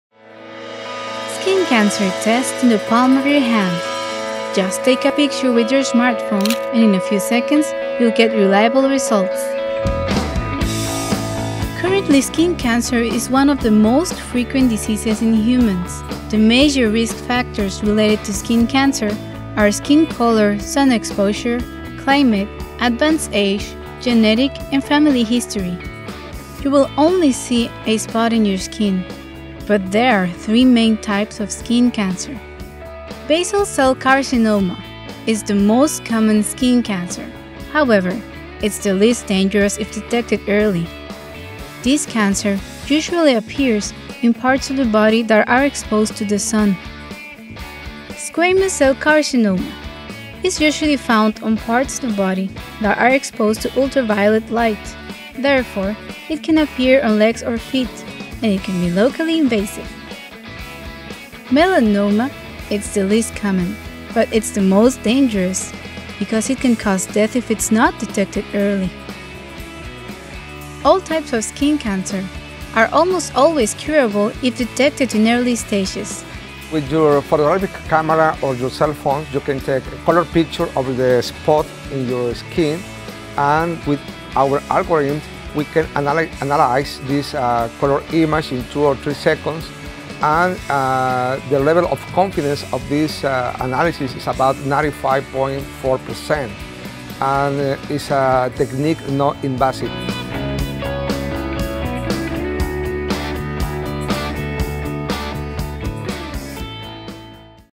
Image Processing and Pattern Recognition
In just a few seconds you can find out if you suffer from skin cancer, thanks to a research conducted at CICESE  by Dr. Josué Álvarez in the Optics Department.

En tan sólo unos segundos puedes saber si padeces cáncer de piel, gracias a una investigación realizada en el CICESE, a cargo del doctor Josué Álvarez, investigador del Departamento de Óptica.